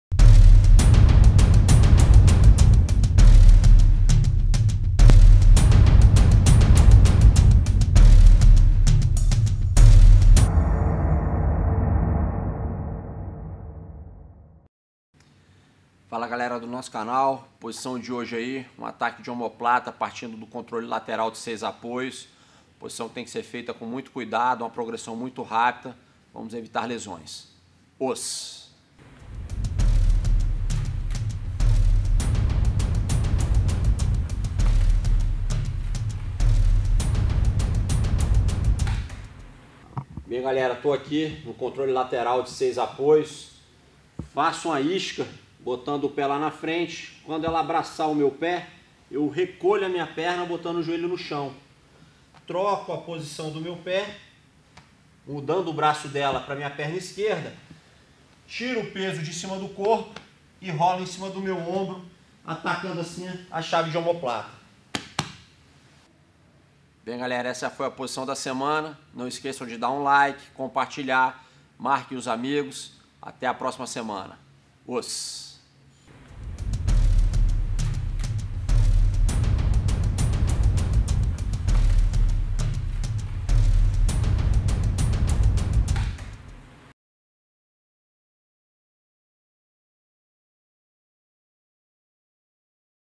Omoplata - partindo do controle lateral
Finalização com chave de omoplata partindo do controle lateral de 6 apoios.